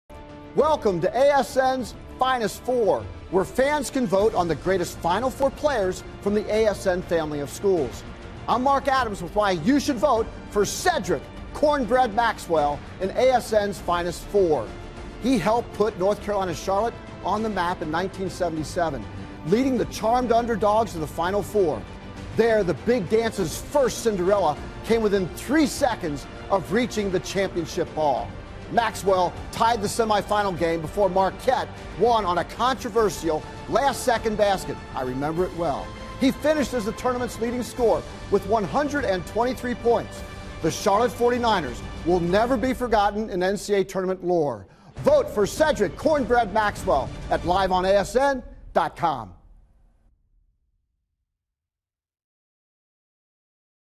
Cedric Maxwell for ASNs Finest Four
Here's why you should vote for Charlotte's Cedric Maxwell (now UTEP) in ASN's Final Four.